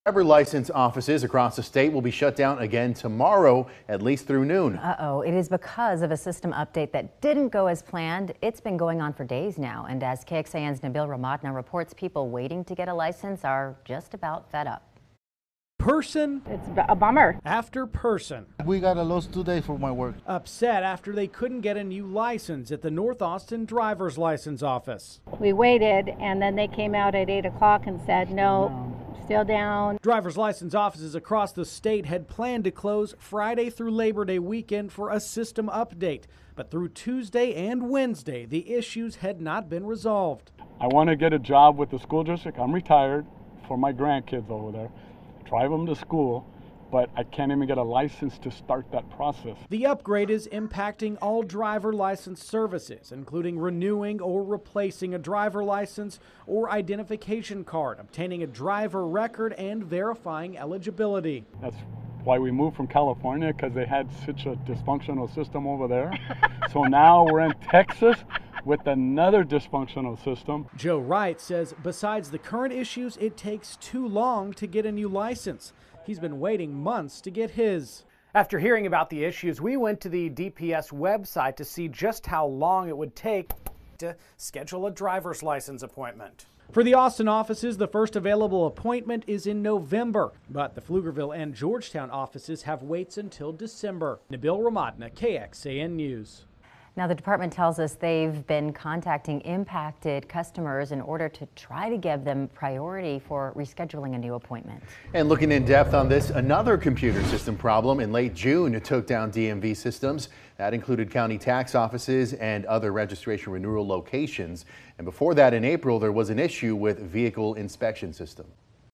Need a driver’s license? Get in line
The Texas Department of Public Safety announced all driver’s license offices were closed Wednesday after issues with their updated system continued.